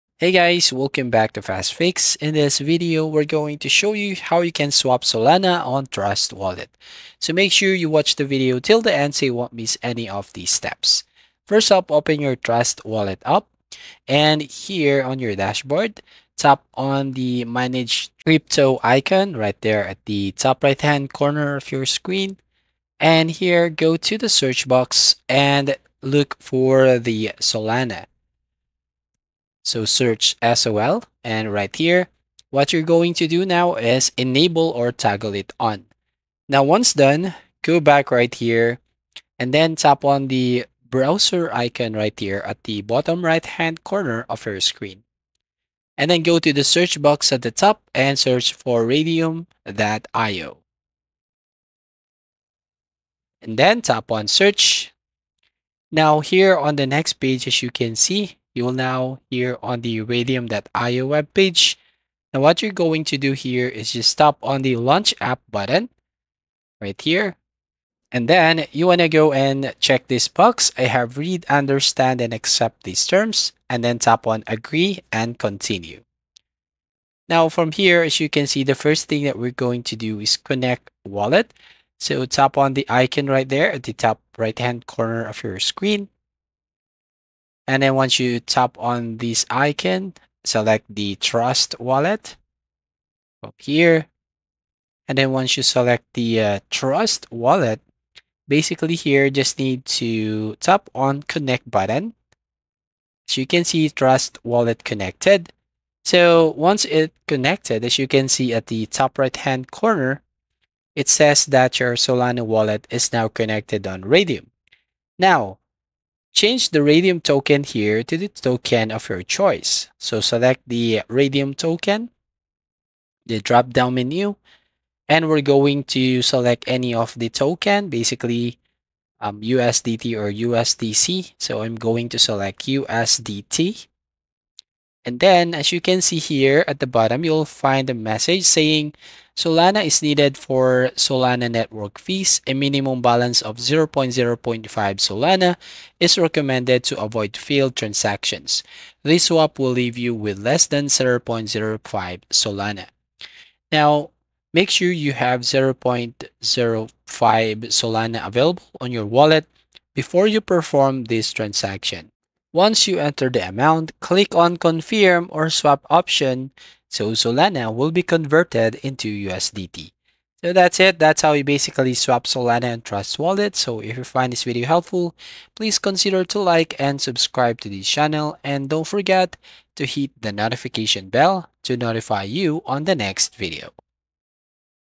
How to Swap SOLANA on TRUST WALLET 2024 (Transfer Solana on Trust Wallet)
How to Swap Solana on Trust Wallet 2024 (Transfer Solana on trust Wallet). In this video I'll show you how to how to buy Solana on trust wallet and swap it. Would you like to change your Solana token for other crypto Currency?The method is very simple and clearly described in the video. Follow all of the steps & you will know how to add Solana crypto on trust wallet and change it for other crypto.